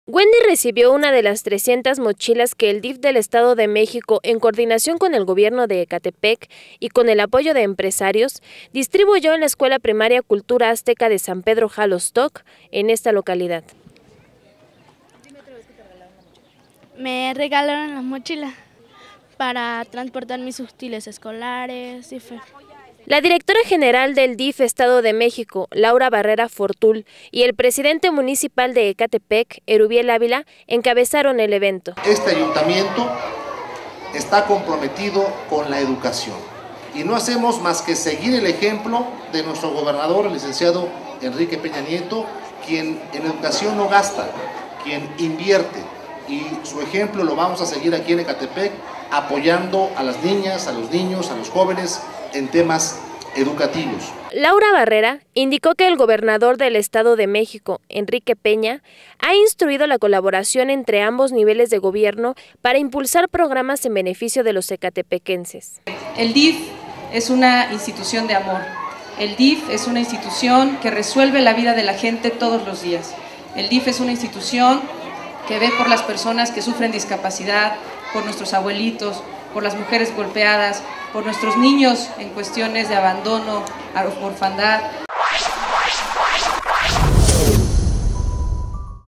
Entregan mochilas a alumnos de Ecatepec
El DIF del Estado de México, empresarios y el gobierno de Ecatepec distribuyeron mochilas escolares.